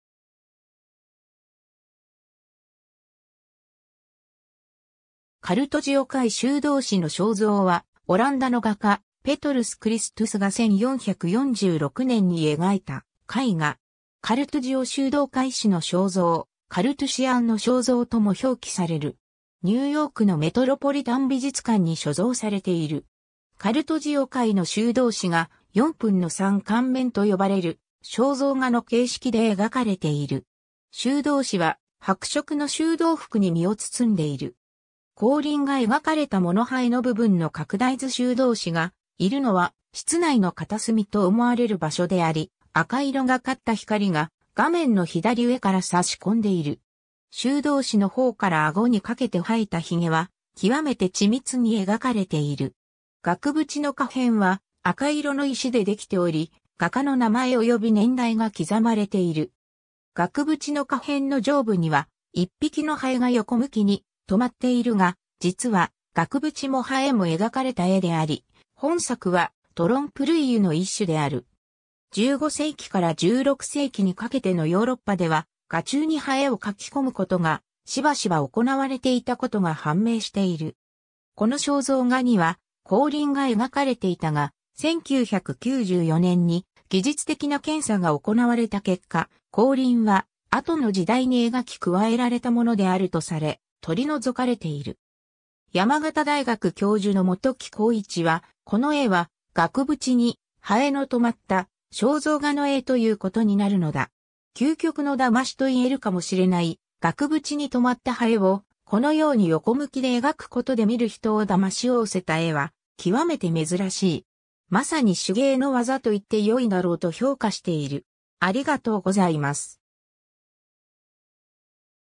カルトジオ会修道士の肖像
『カルトジオ会修道士の肖像』（カルトジオかいしゅうどうしのしょうぞう、オランダ語: Portret van een kartuizer, 英語: Portrait of a Carthusian）は、オランダの画家ペトルス・クリストゥスが1446年に描いた絵画。
 『カルトゥジオ修道会士の肖像』『カルトゥシアンの肖像』とも表記される。
ニューヨークのメトロポリタン美術館に所蔵されている。
 カルトジオ会の修道士が、四分の三観面と呼ばれる肖像画の形式で描かれている。
修道士は、白色の修道服に身を包んでいる。
 光輪が描かれたもの ハエの部分の拡大図 修道士がいるのは、室内の片隅と思われる場所であり、赤色がかった光が画面の左上から差し込んでいる。
 修道士の頬から顎にかけて生えた髭は、極めて緻密に描かれている。
 額縁の下辺は、赤色の石でできており、画家の名前および年代が刻まれている。
額縁の下辺の上部には、1匹のハエが横向きに止まっているが、実は、額縁もハエも描かれた絵であり、本作は、トロンプ・ルイユ（騙し絵）の一種である。
 15世紀から16世紀にかけてのヨーロッパでは、画中にハエを描き込むことが、しばしば行われていたことが判明している。
 この肖像画には、光輪が描かれていたが、1994年に技術的な検査が行われた結果、光輪は後の時代に描き加えられたものであるとされ、取り除かれている。
 山形大学教授の元木幸一は、「この絵は『額縁に蝿のとまった肖像画』の絵ということになるのだ。究極のだましといえるかもしれない」「額縁にとまった蝿をこのように横向きで描くことで見る人をだましおおせた絵は、きわめて珍しい。まさに至芸の技といってよいだろう」と評価している。